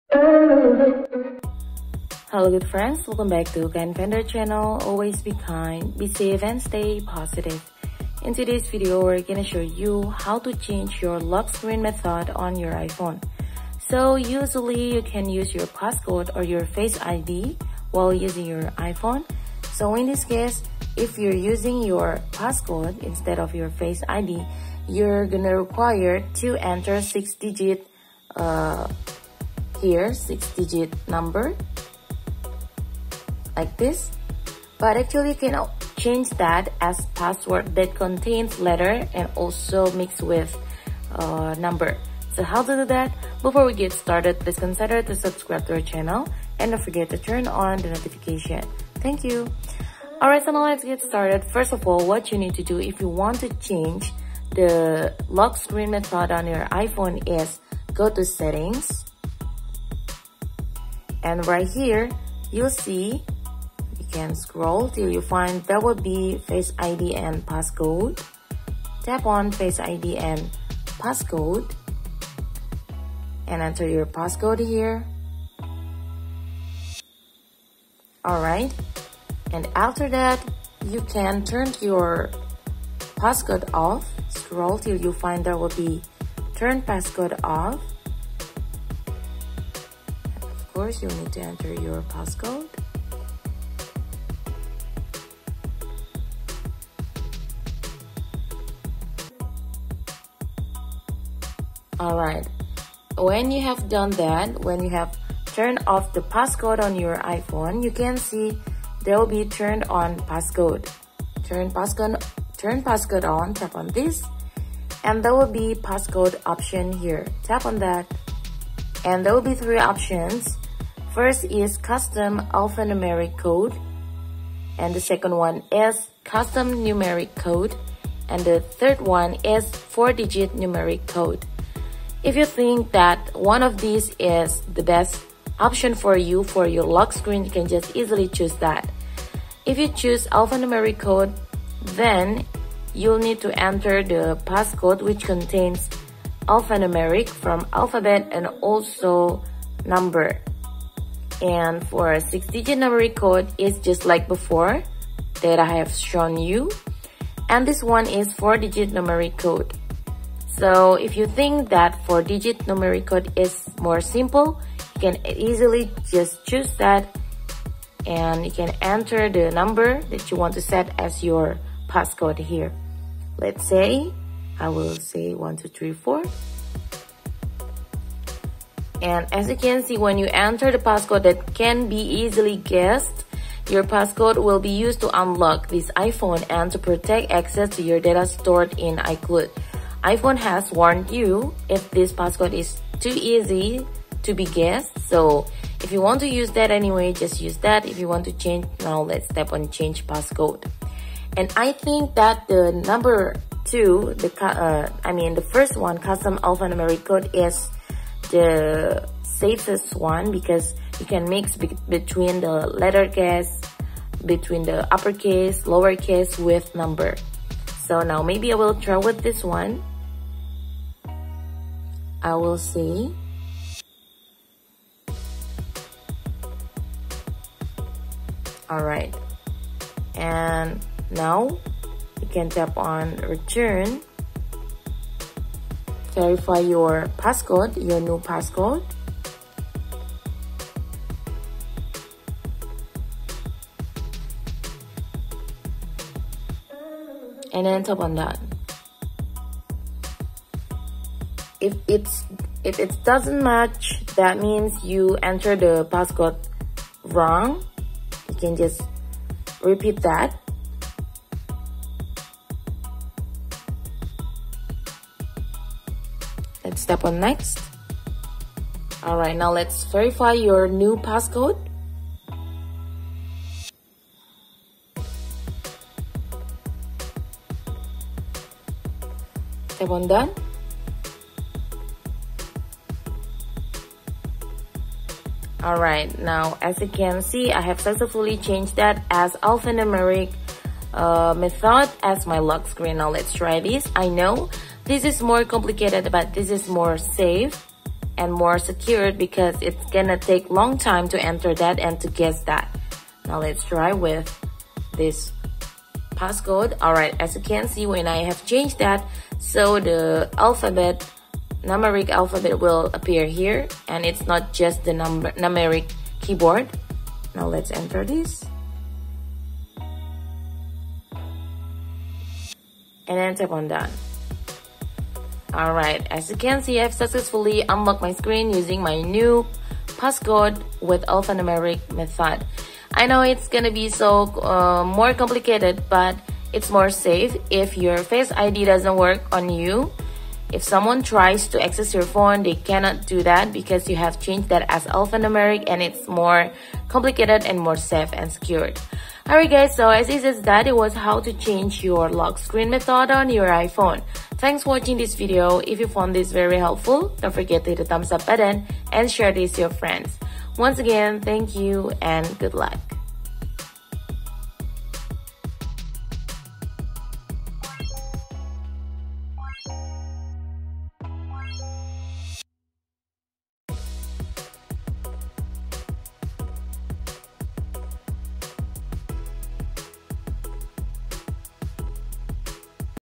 How To Change Passcode Method On iPhone
How to Change Lock Screen Method On iPhone

This video shows you how to Change Passcode Method On iPhone

If you want to know how to Change Passcode Method On iPhone please follow the steps in this video carefully.

Sincerely
Kind Finder